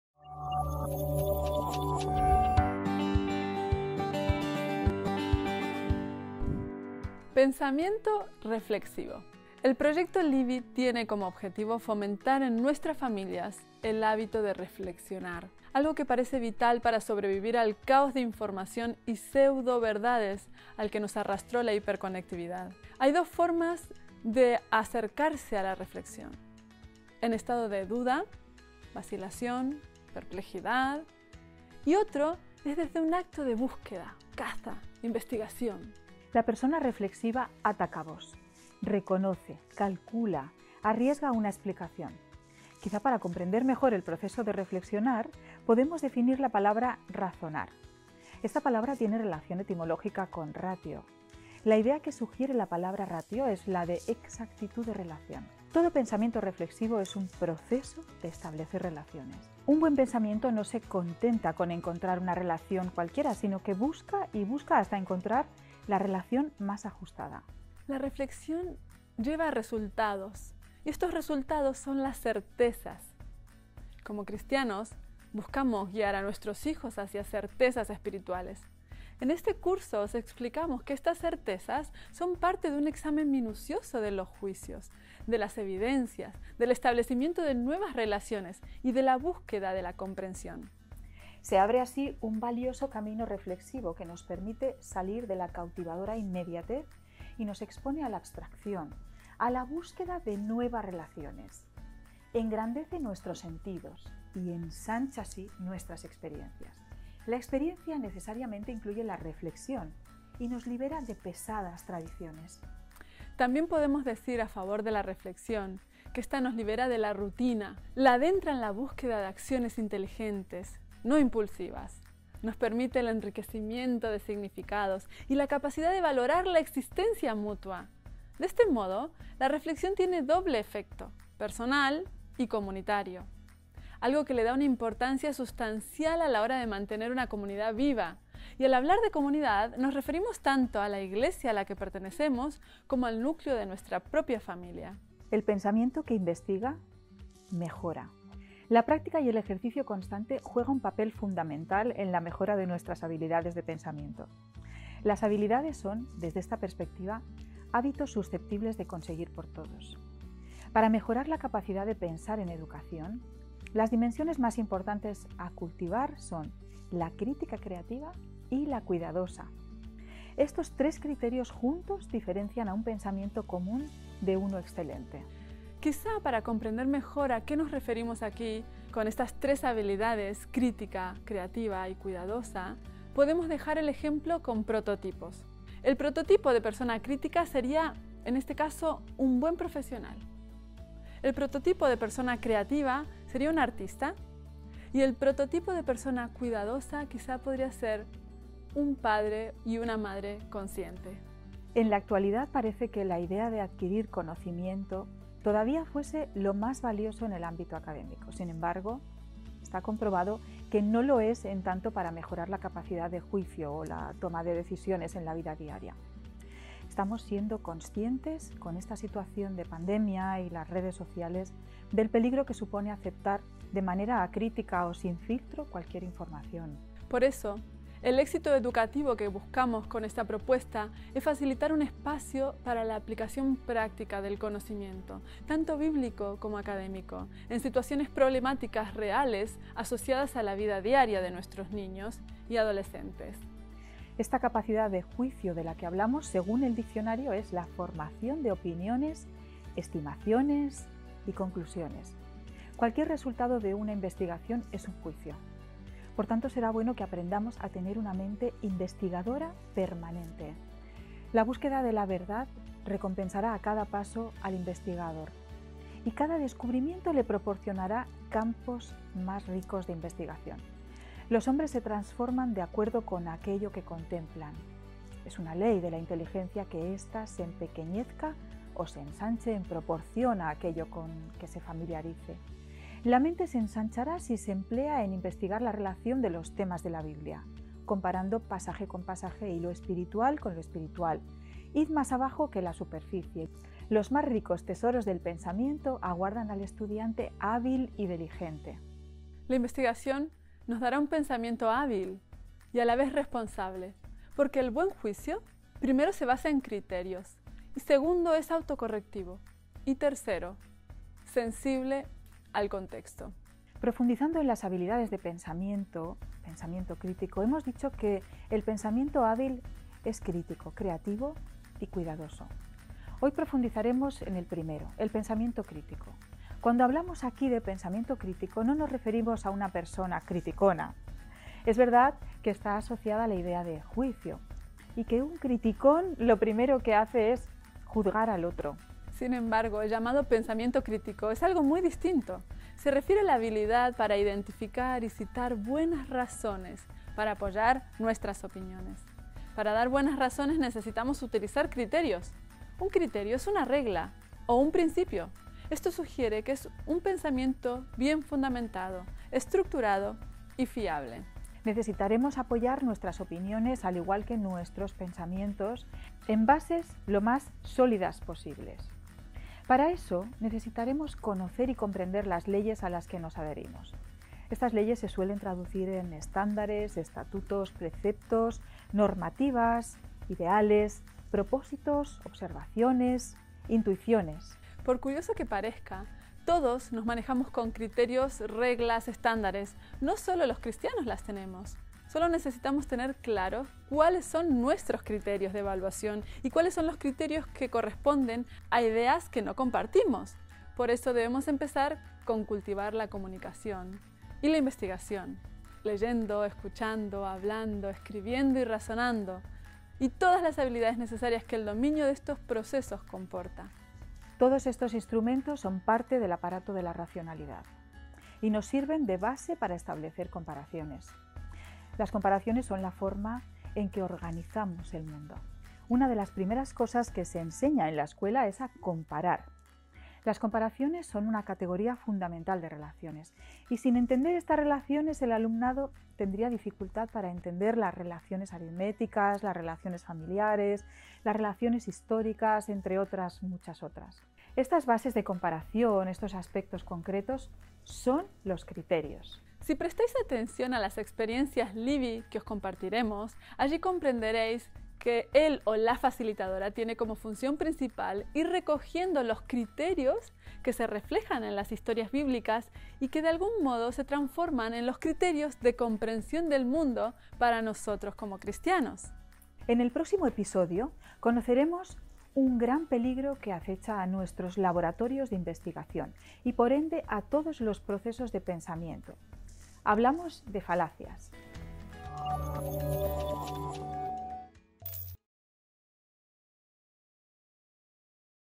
LIBI - Tema 03 - Cultivar en la infancia un pensamiento reflexivo
Mira el video. Toma apuntes. Completa los ejercicios de autoevaluación.